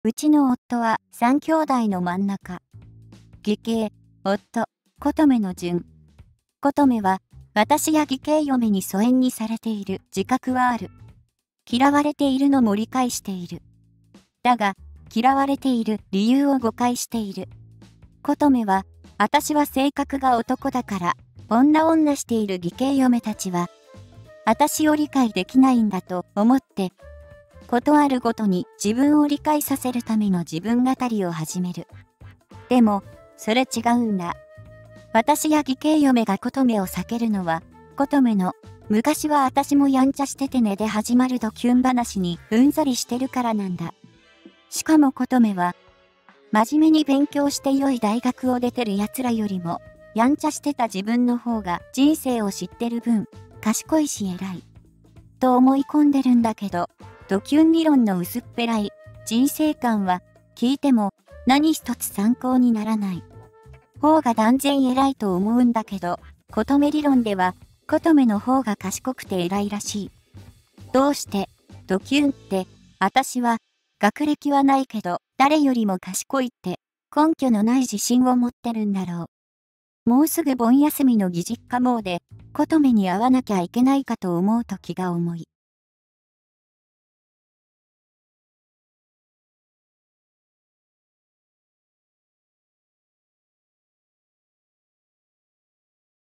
コトメは私や義兄嫁に疎遠にされている、嫌われていると理解している。だけど私（誤解している…）【痛快・スカッとジャパン】
※動画音楽
　LAMP BGM　様